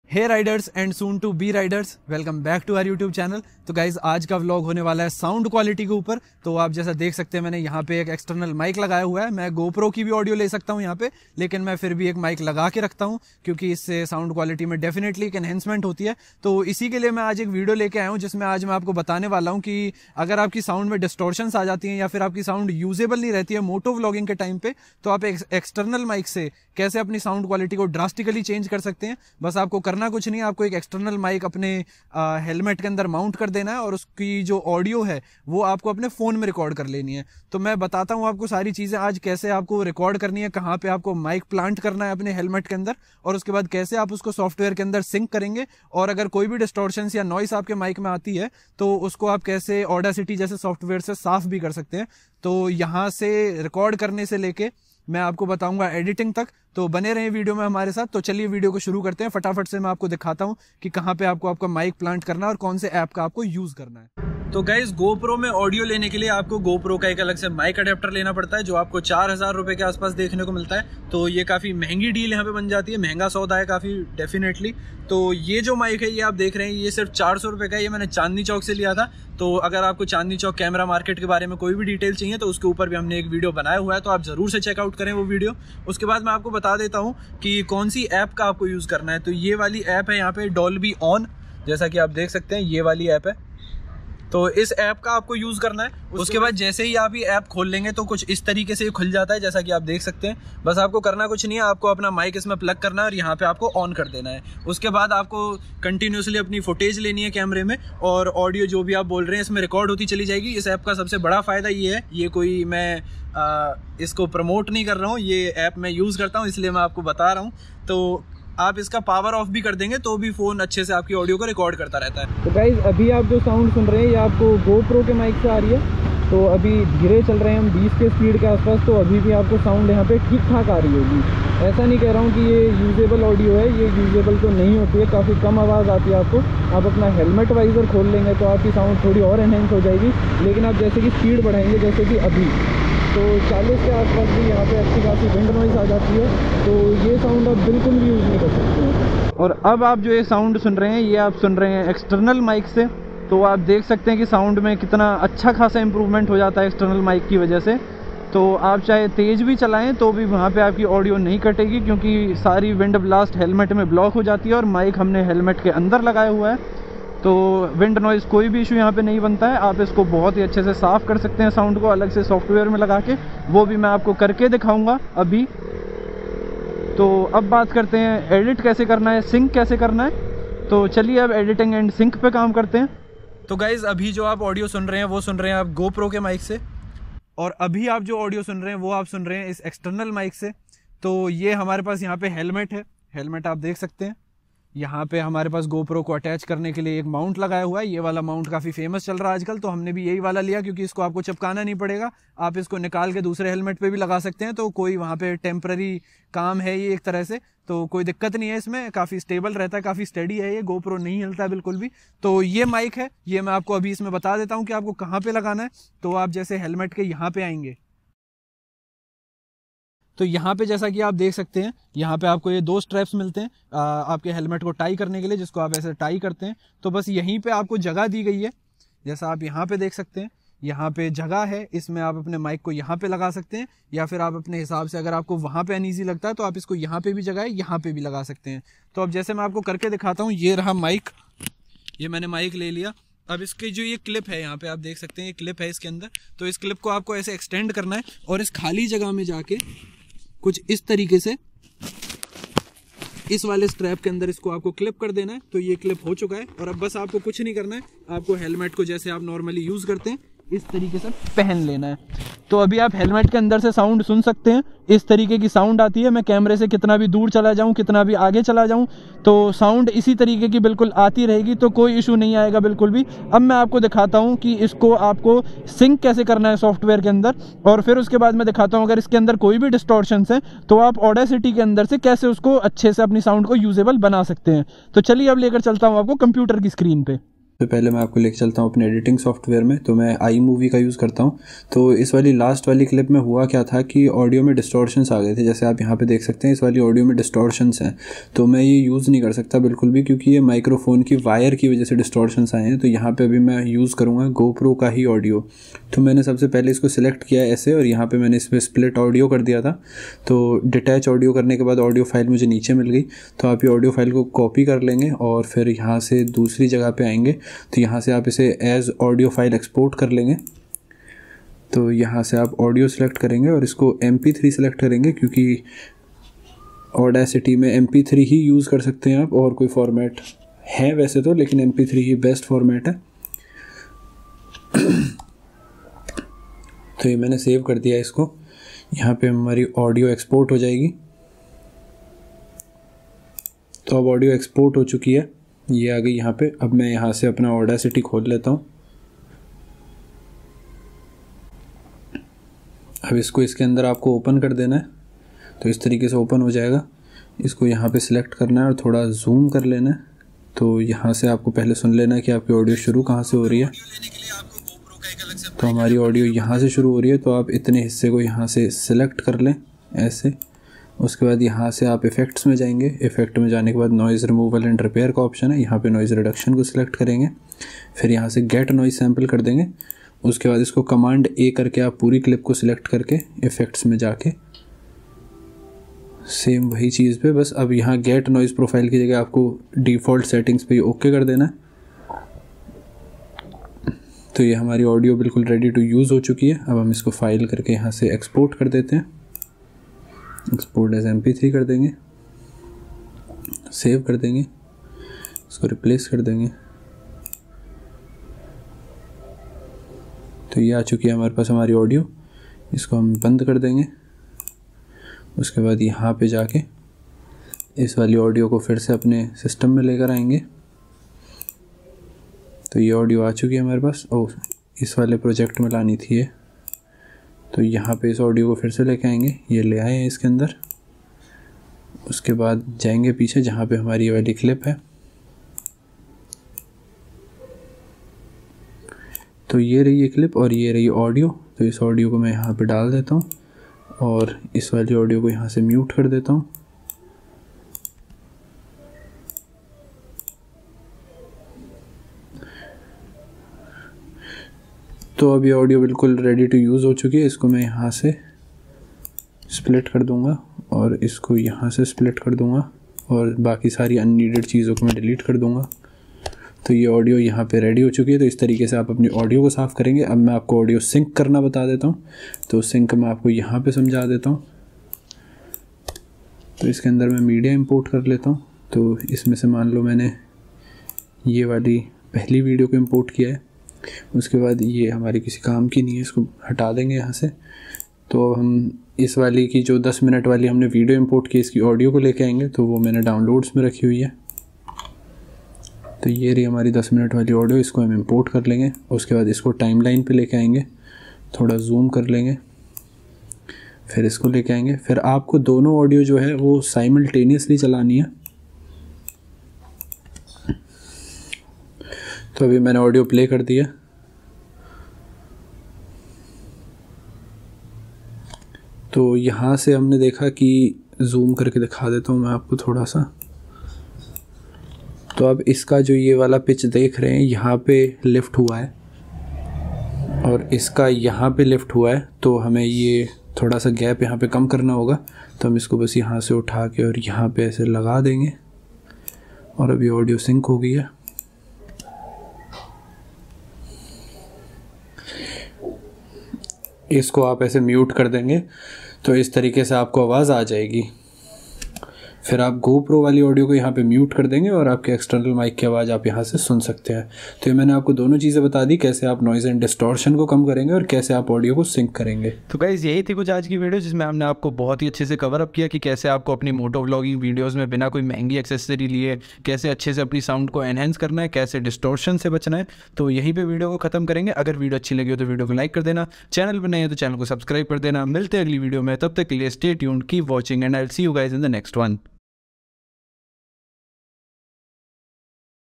How To Motovlog | No GOPRO Adapter | Audacity Editing & Syncing
In this video, we'll go over the basics of sound quality for video motovlogging. We'll cover topics like mic selection, audio recording, editing, post-production and how to motovlog. So if you are struggling to motovlog then this video is the one stop solution where I will tell the best method to motovlog without GoPro adapter or any other expensive accessories.

If you're struggling to record sound quality that's good enough for your YouTube videos, then this video is for you! We'll go over the basics of sound quality for video motovlogging and help solve all your problems. By the end of this video, you'll be able to record sound quality that's good enough for your YouTube videos!